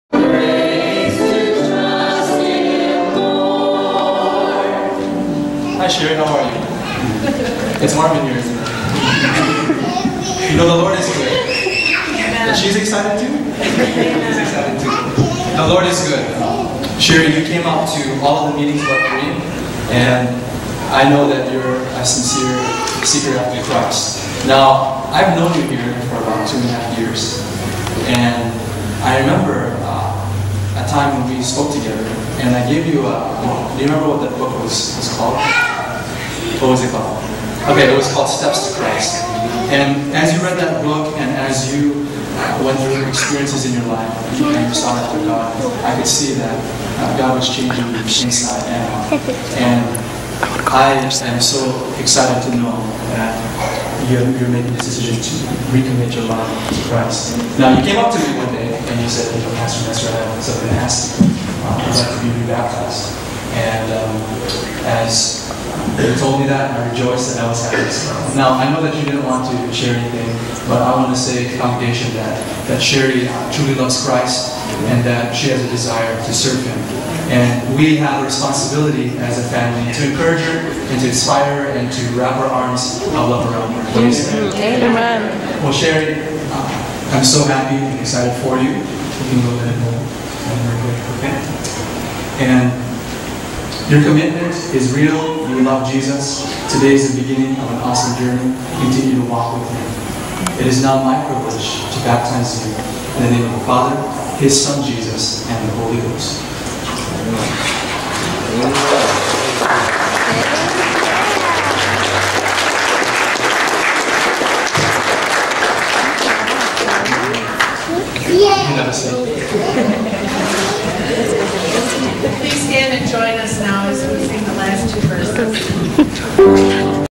My Baptism
chyeahh shawty! My baptism! I'm not sure why the date is on there or why it's blurry on some of the parts. It wasn't on my Sony handy-cam...it happened when I was editing it, and the date is actually supposed to be May 1st not May 2nd. lol
-at the Madison east Seventh-day Adventist church in the state where happy cows come from! pssssstt...that's Wisconsin, my California friends. ^-^ mwahaha